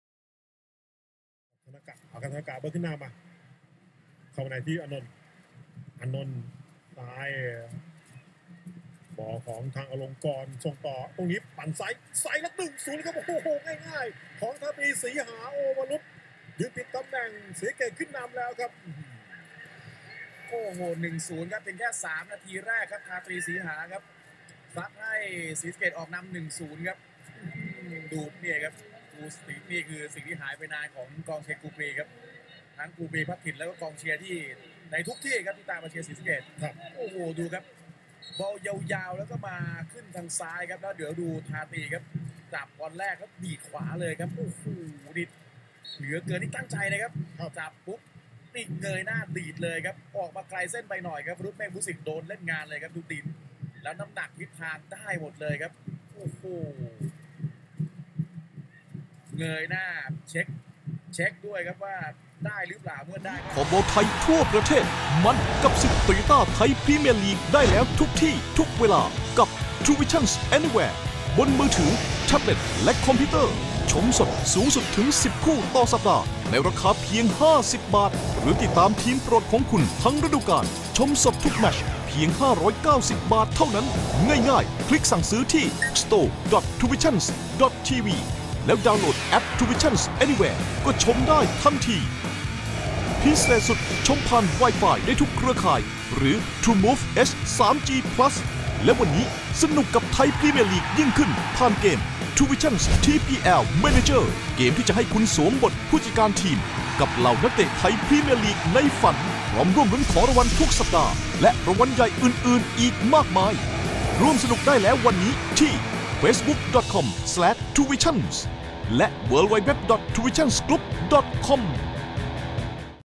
TPL 2014 - Highlight - BUFC 0-1 SSKFC (08-03-2014)
BUFC 0-1 SSKFC
นาทีที่ 3  ธาตรี สีหา   SSKFC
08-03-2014